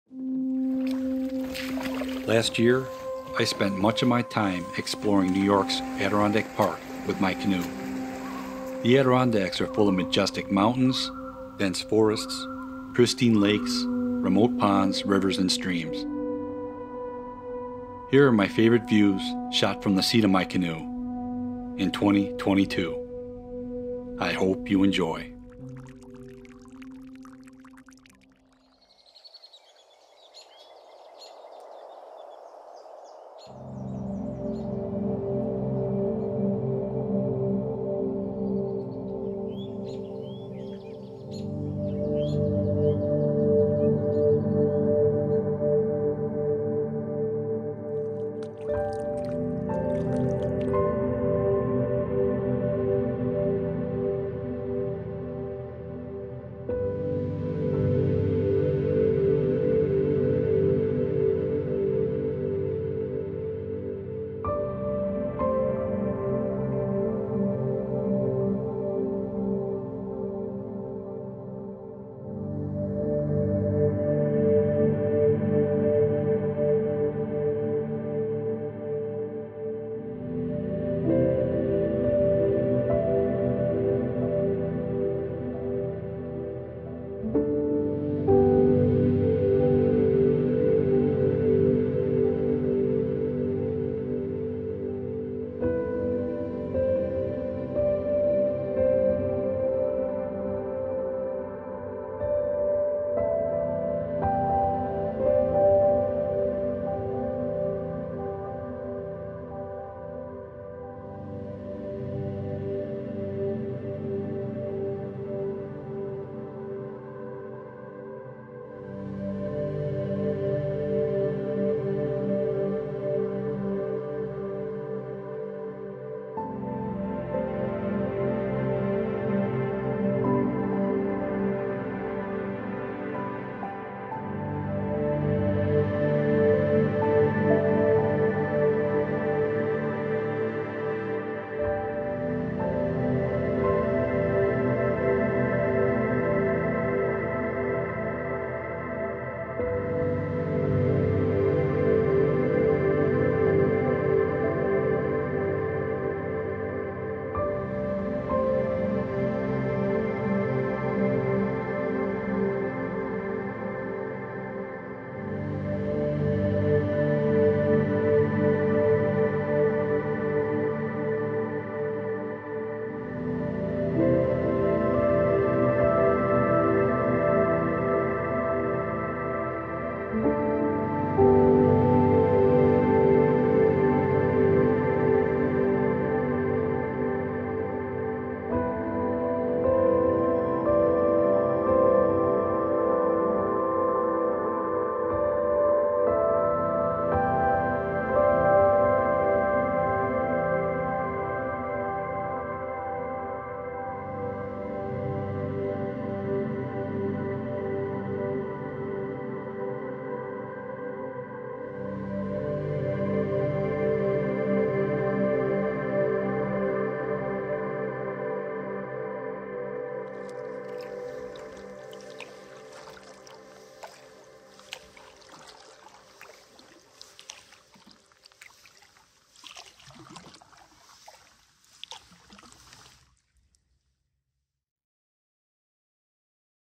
The Best Adirondack Canoe Views | Saranac Lakes | Boreas Ponds | Saint Regis | Fish Creek
I hope you enjoy my favorite Adirondack paddling views... shot from the seat of my canoe... in 2022. All of these clips were taken in the Adirondack Park during the 2022 paddling season. The views include Santanoni Brook, Henderson Lake, Boreas Ponds, the West Branch of the Ausable River, Lake Eaton, the Bog River Flow, Rollins Pond Outlet, Fish Creek, and a few Saint Regis Canoe Area waterways including Bog Pond, Turtle Pond Outlet, and Little Long Pond East. I wish you a safe and enjoyable paddling season.